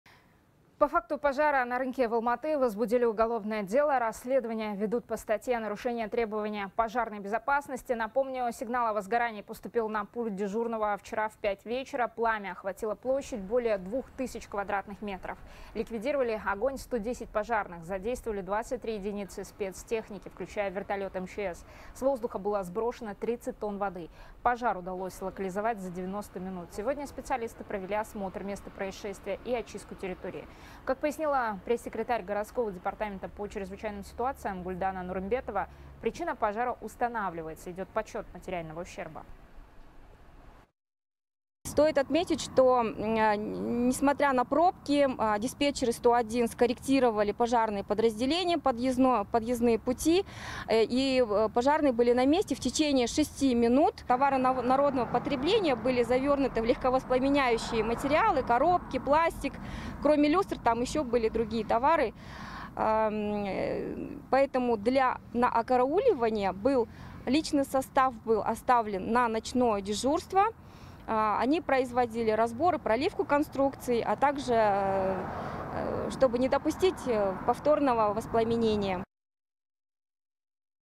Пожар на рынке в Алматы: возбуждено уголовное дело
По факту пожара на рынке в Алматы возбудили уголовное дело, передаёт «24KZ».